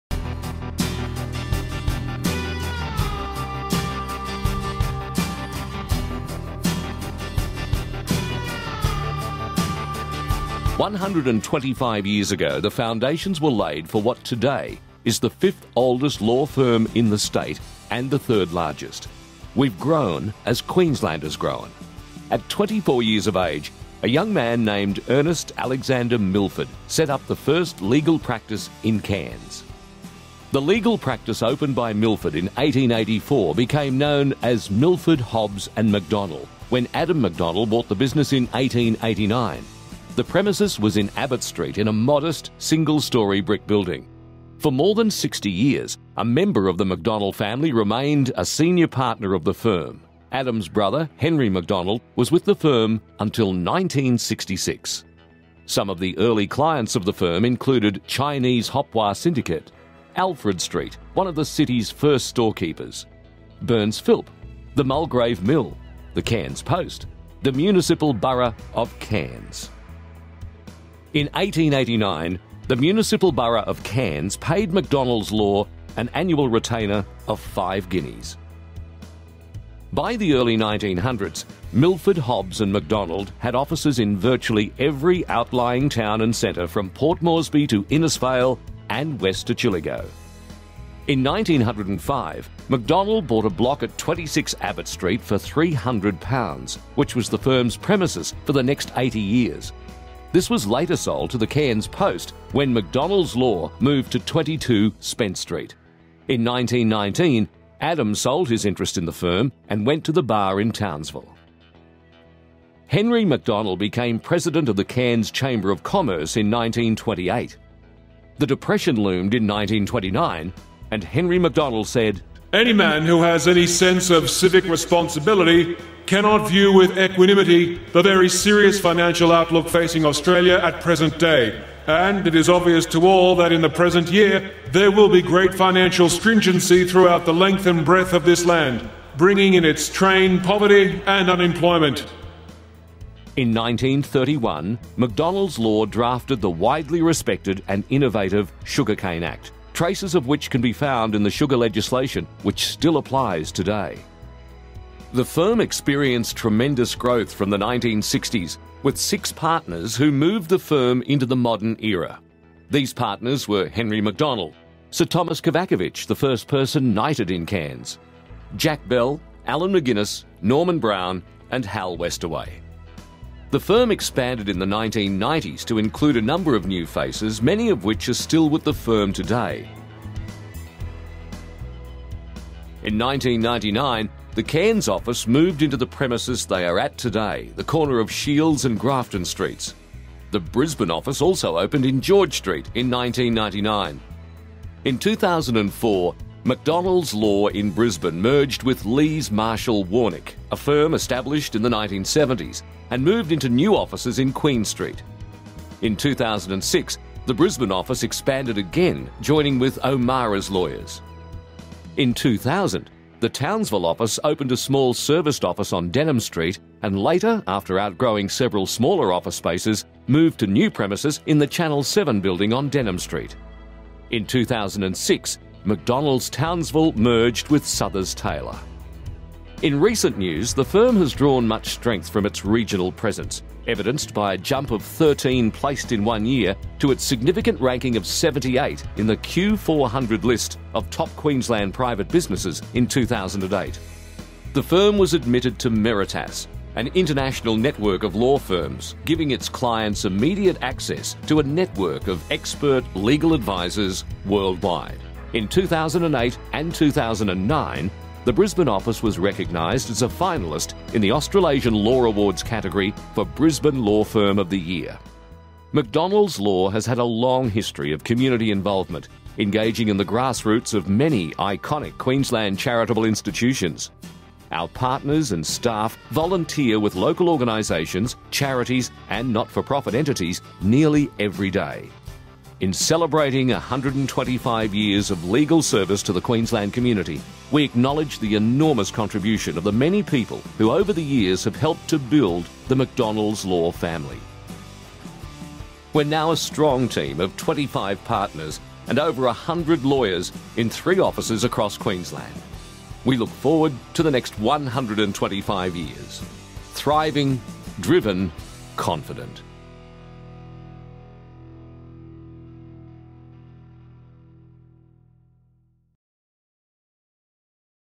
MacDonnells Law - 125 Years in Queensland
MacDonnells Law celebrates its 125th Anniversary in 2009 as the fifth oldest law firm in Queensland. This video was produced by MacDonnells Law in conjunction with Enso Studios to commemorate this milestone. With offices in Brisbane, Townsville and Cairns, MacDonnells Law is a full service law firm in both the commercial and litigation space. In 2008, the firm joined Meritas, a worldwide network of law firms and now moves forward with confidence, thriving and ready to provide value to its clientele across the world.